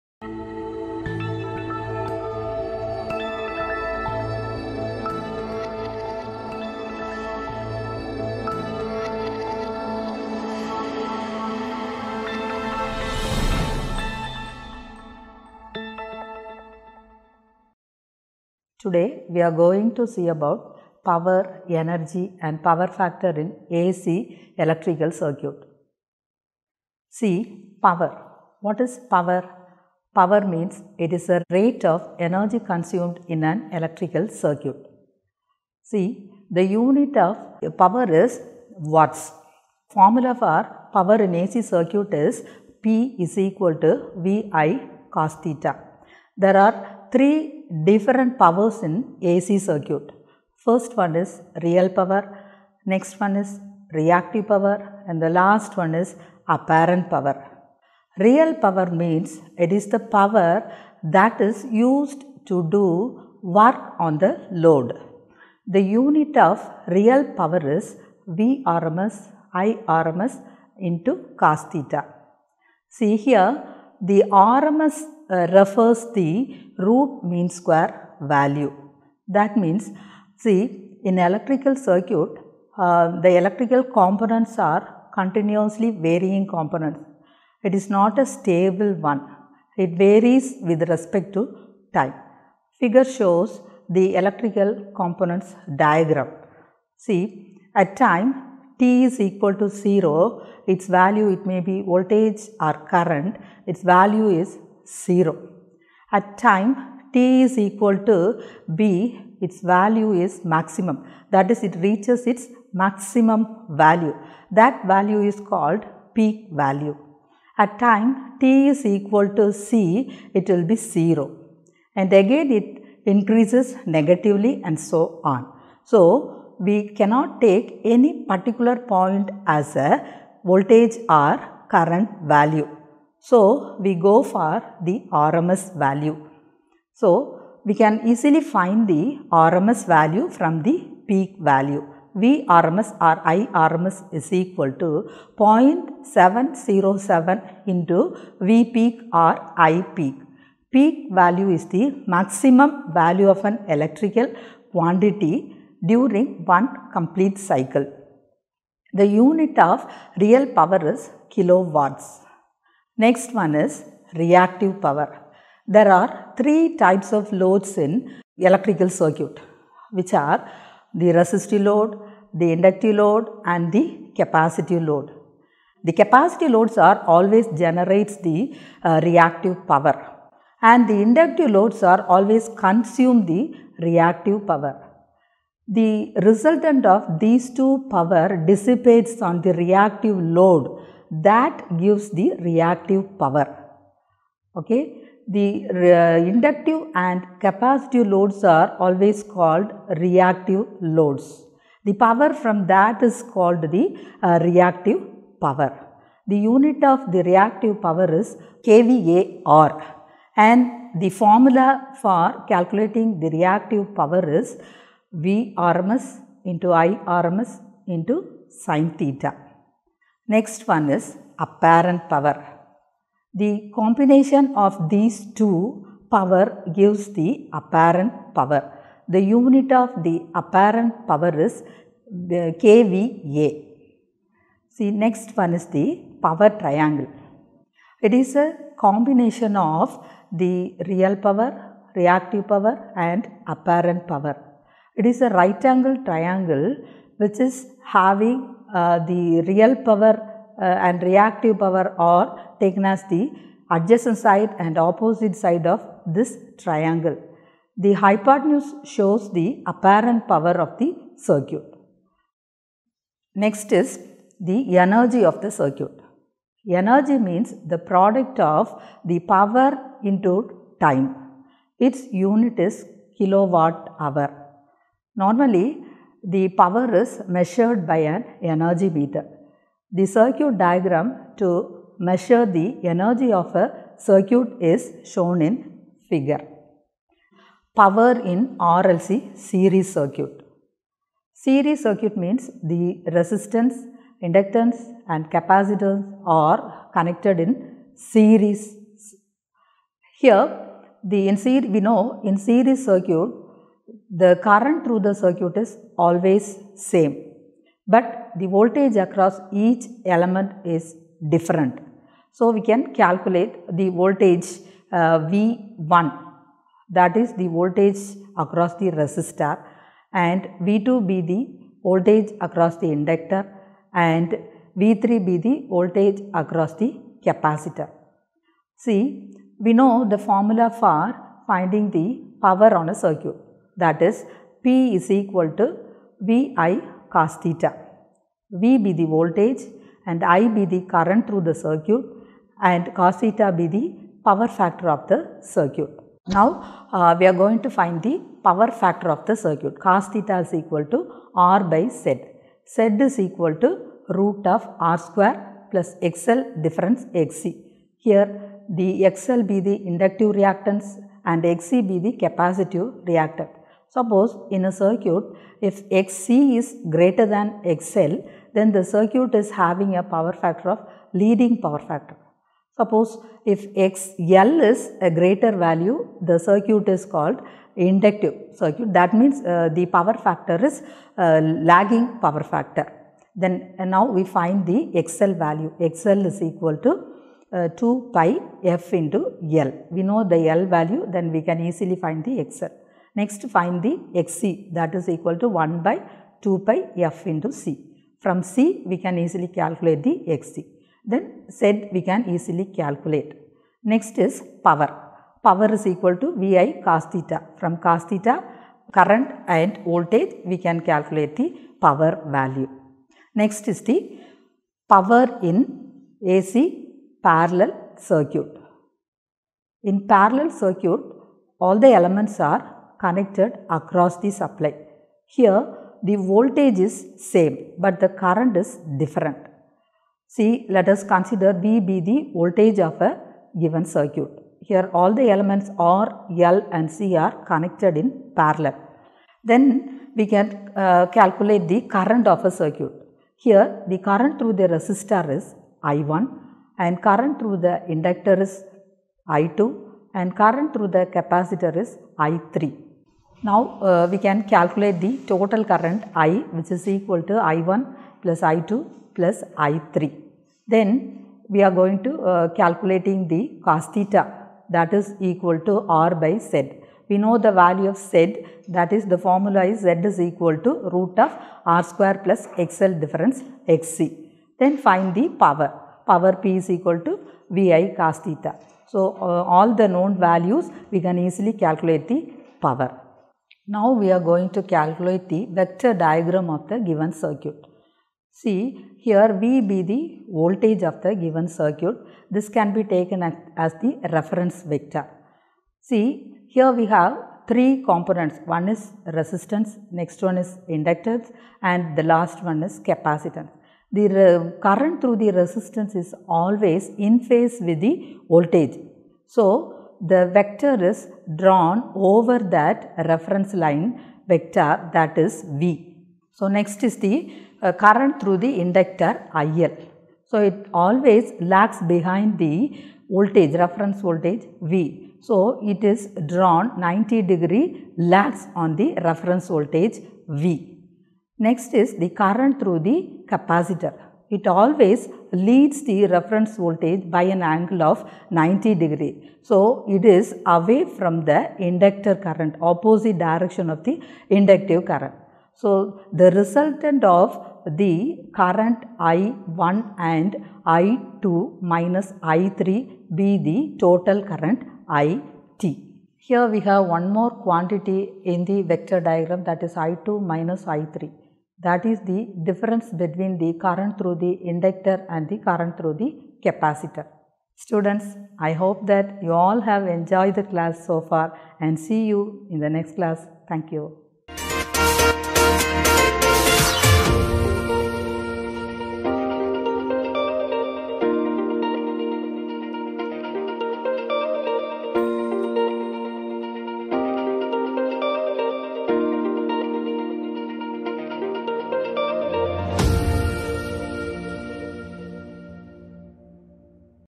Electrician | Power energy and power factor in ac electrical circuit
This Video tells us about power factor used in an electrical circuit